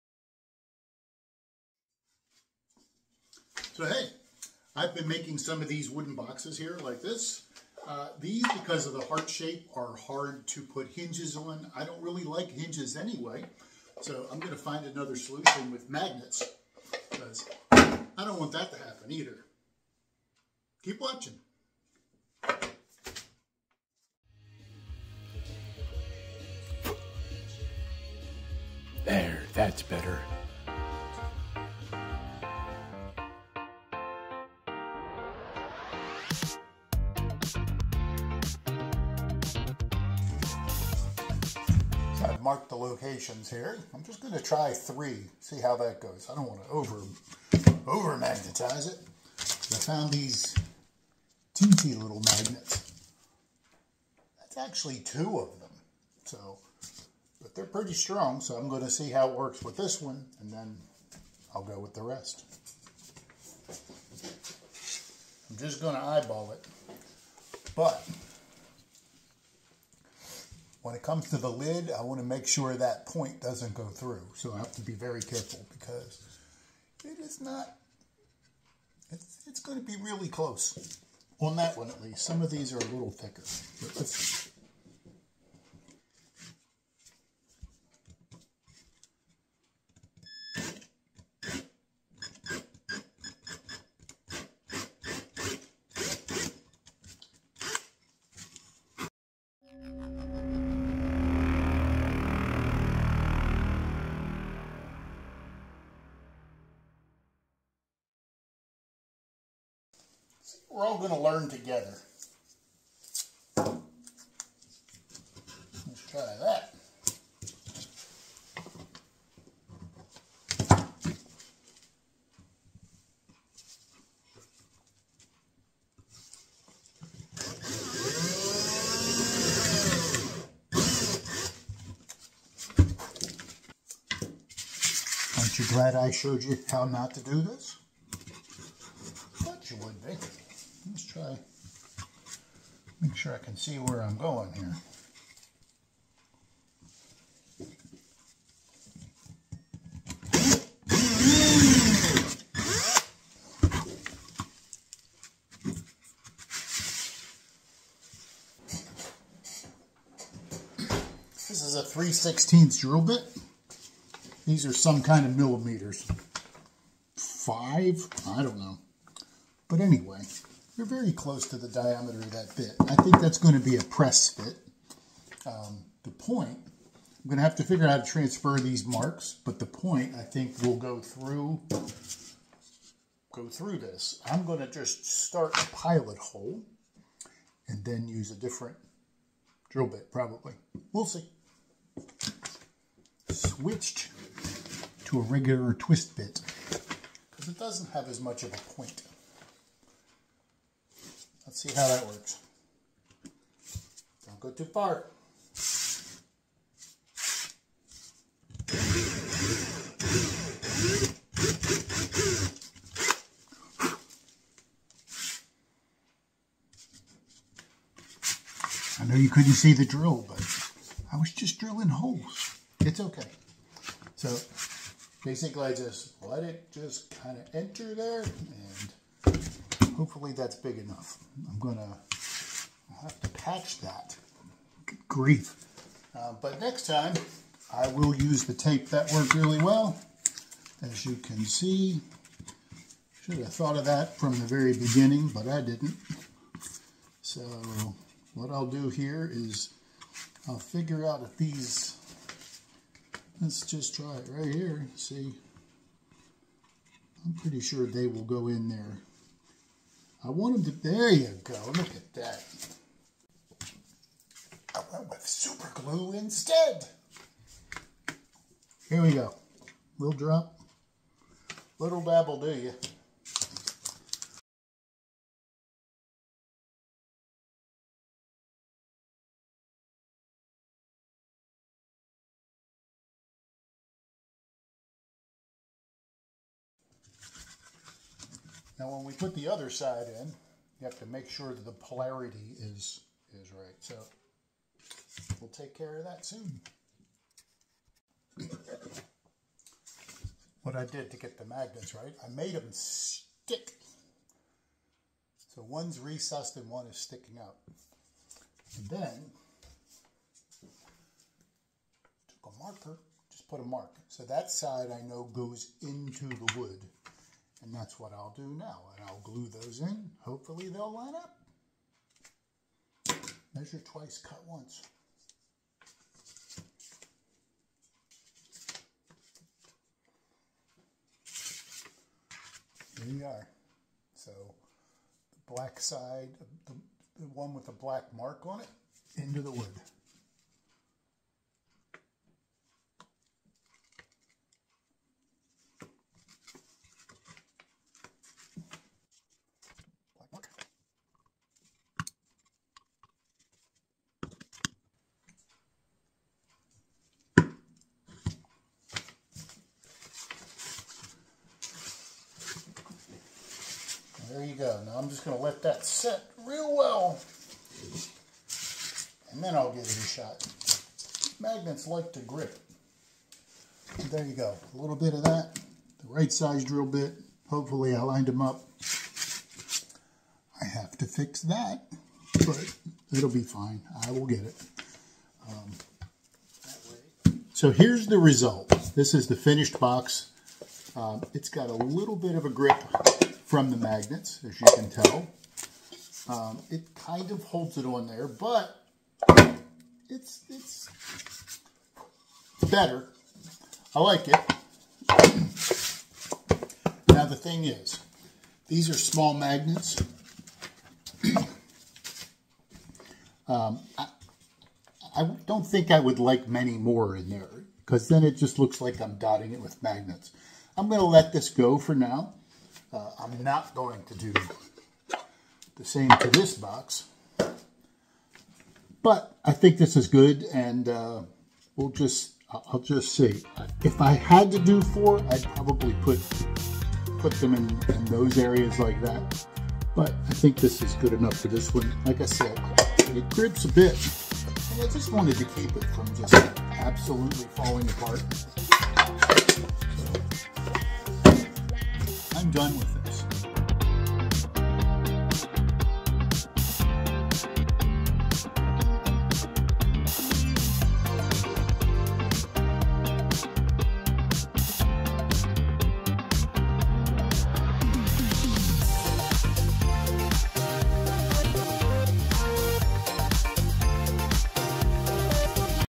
Using Tiny Magnets to Keep the Lid on My Heart-Shaped Lidded Box
This is not the most complex video you could watch today, but if you make these boxes, then this might be just the thing for you. I've seen people use a small dowel or screw to fix the lid to the box so it swivels, and I've seen hinges awkwardly mounted to the back. I don't like these methods, so I went for little magnets. They won't absolutely secure the lid, but it sticks it down just enough for me. I hope you like it!